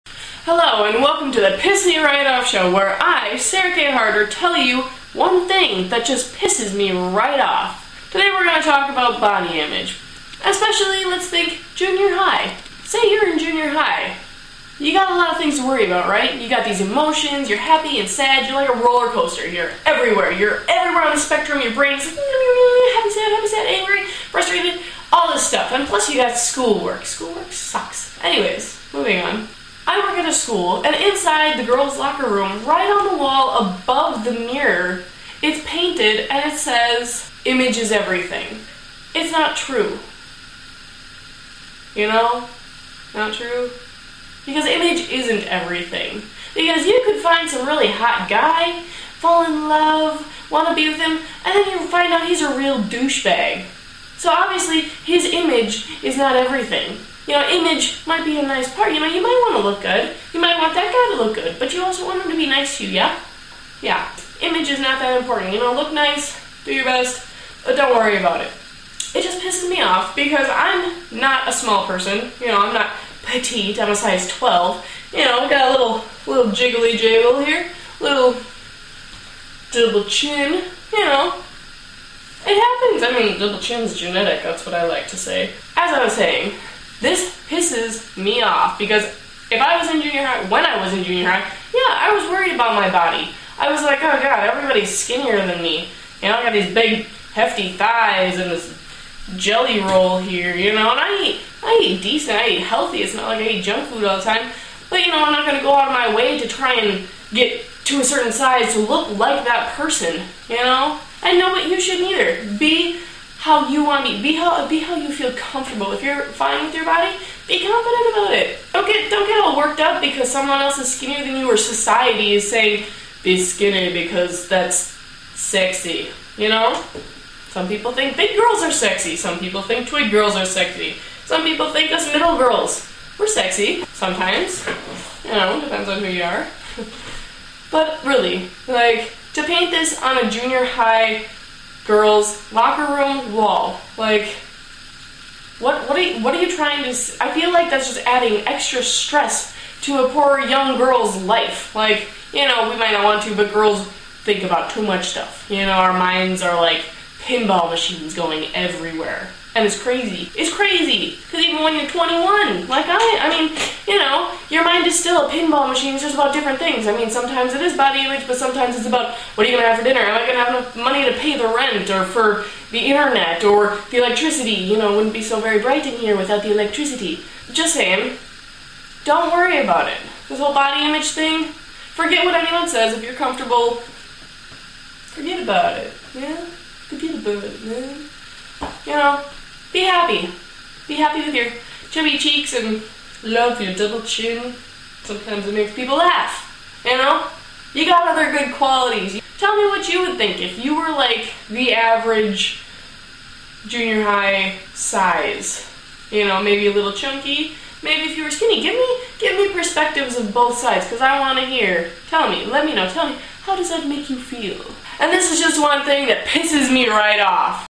The Piss Me Right Off Show Image Is Everything
Me ranting about things that piss me right off. Sorry about the watermark, I was trying out an editing program and that just happened to be there